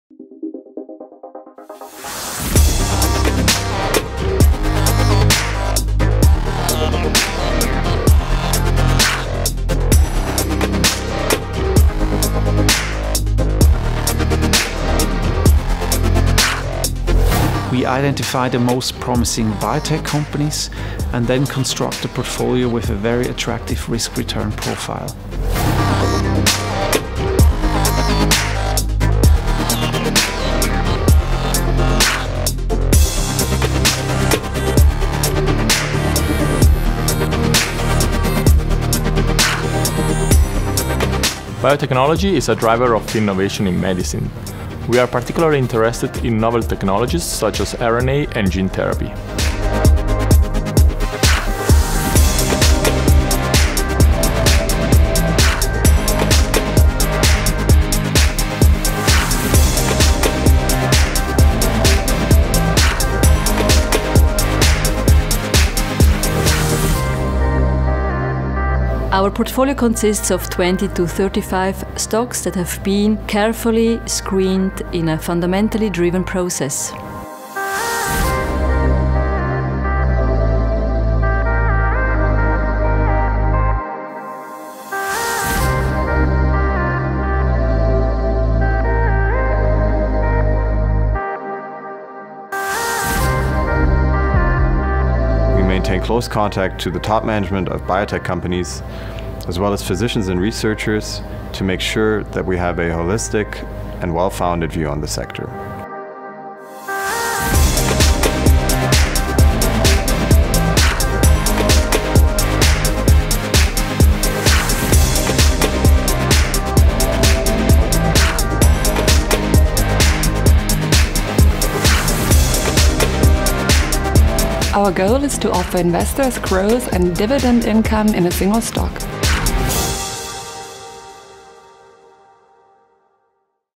25 years of experience in biotech investments
Discover BB Biotech and gain insight into the biotechnology sector and the fundamental analysis process of our management team when selecting investments.